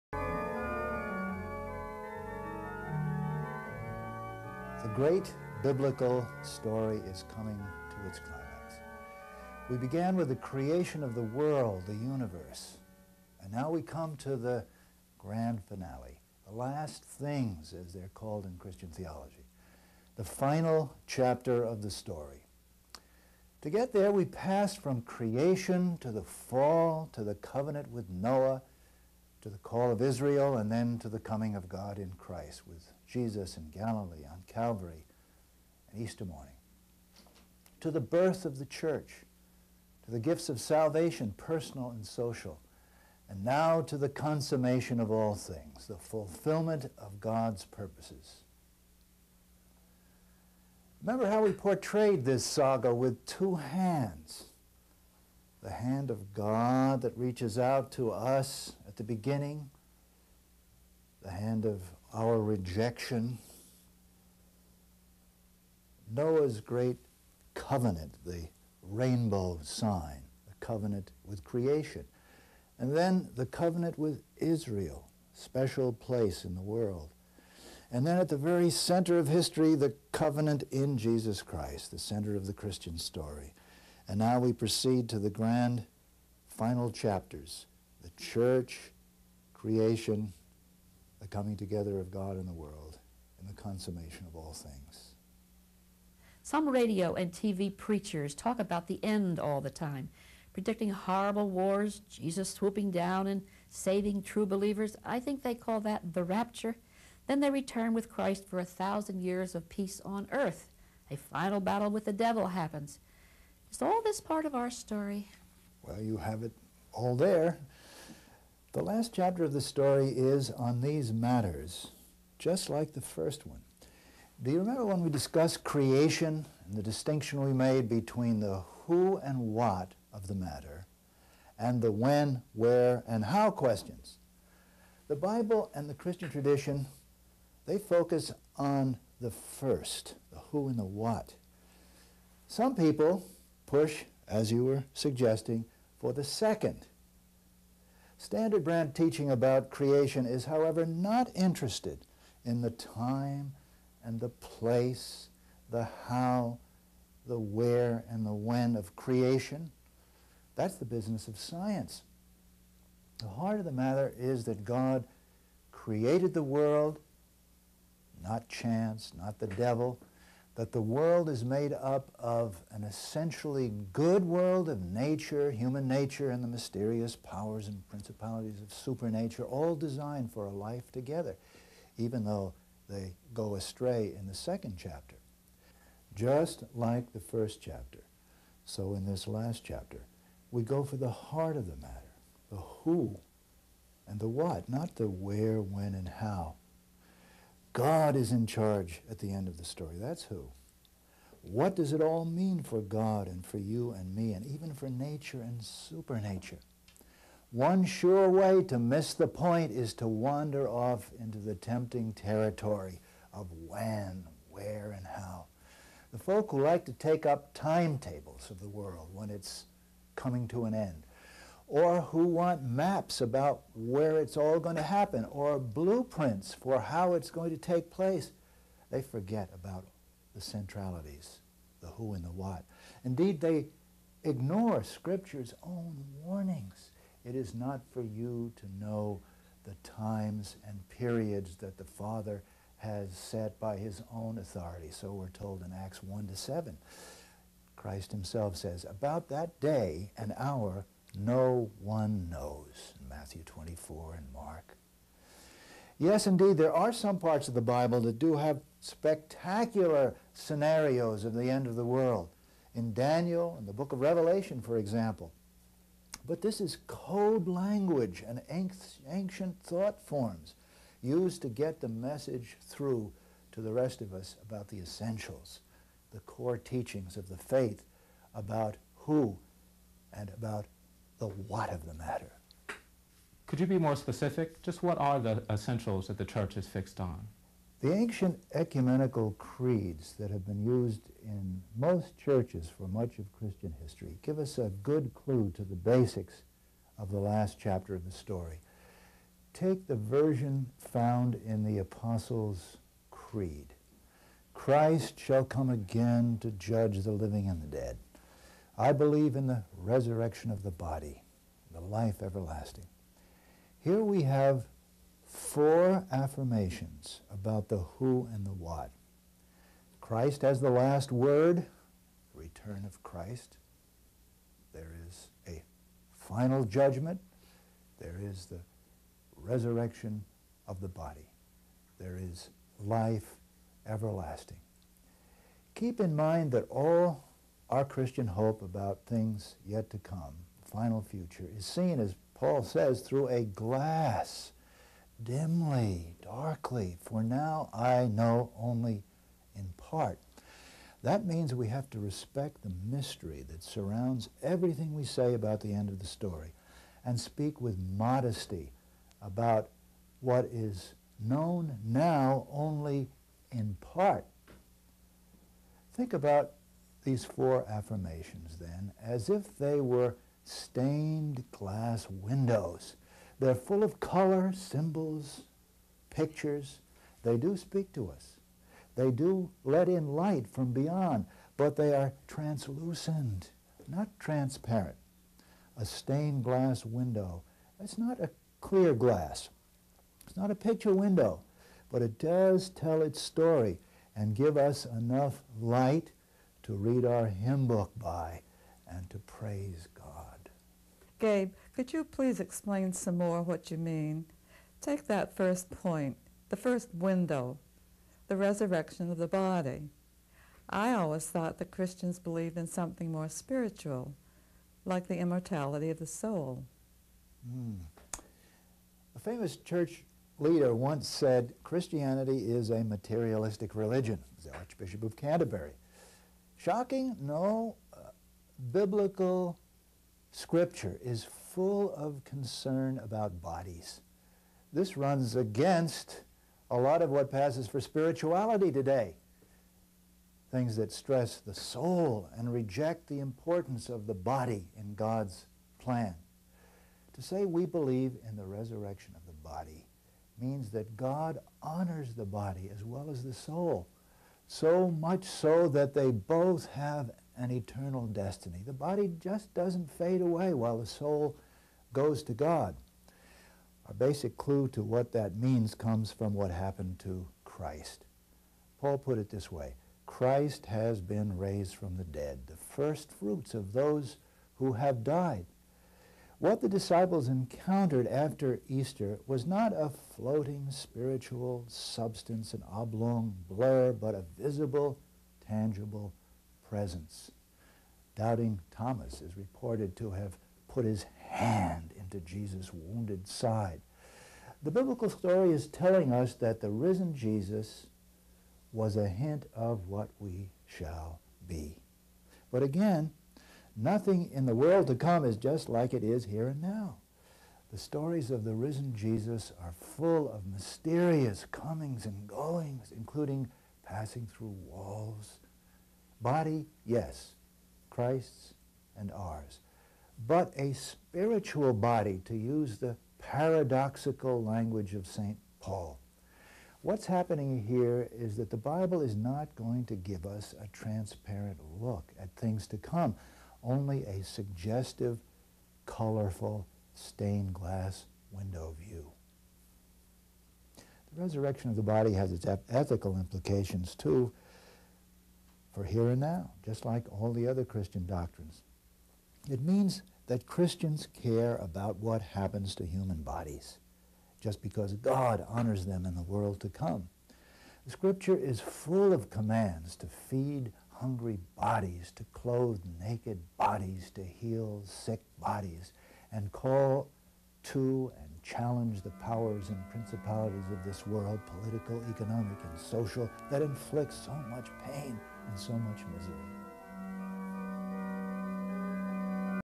Gabriel Fackre's Christian Basics - Lesson 18
Series of lectures on the basics of Christianity given by Dr. Gabriel Fackre while he was a professor at Andover Newton Theological School.